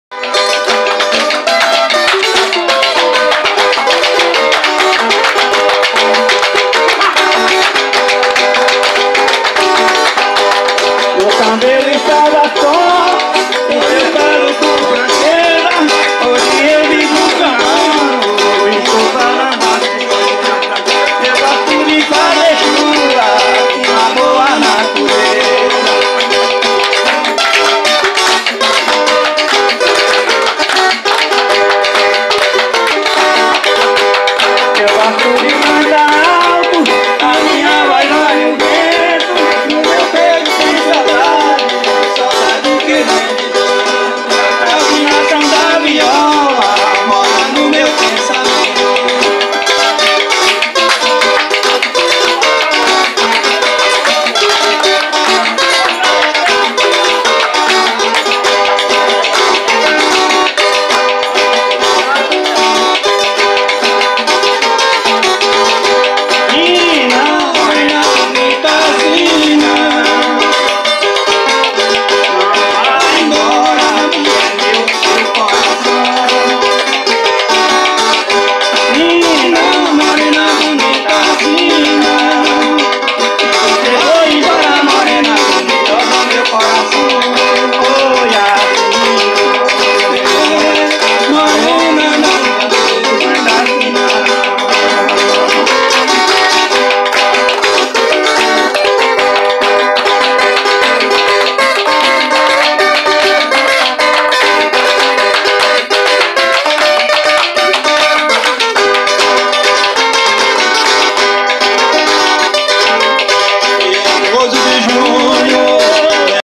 II Encontro de Sambadô. Utinga.Ba - Chapada
Encontro de Sambadô de Utinga BA Chapada Diamantina e região no terreiro da pousada aconchego.